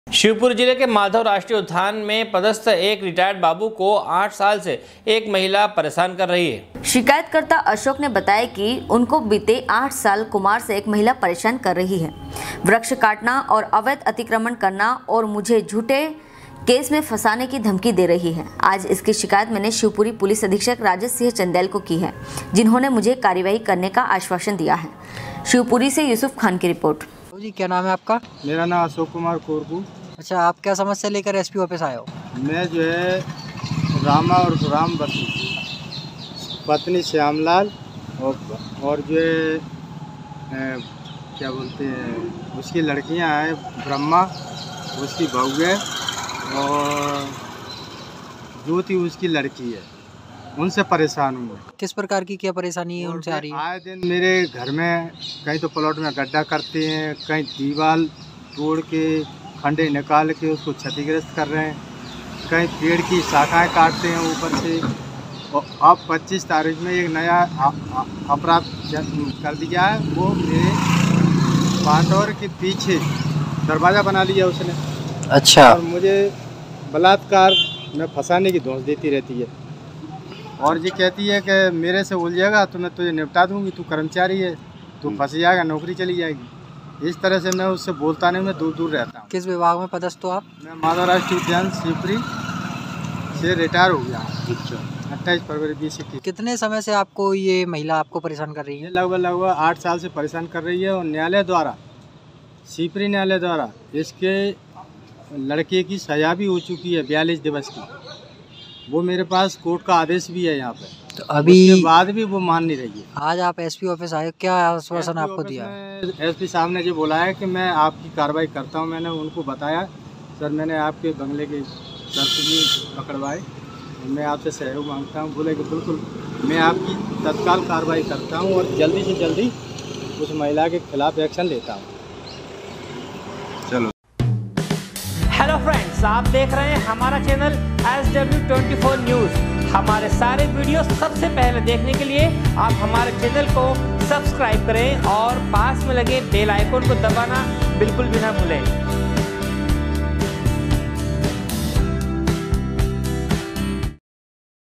SW 24 News Madhyapradesh
शिवपुरी जिले के माधव राष्ट्रीय उद्यान में पदस्थ रहे एक रिटायर्ड बाबू को 8 साल से एक महिला परेशान कर रही है। शिवपुरी से युसूफ खान की रिपोर्ट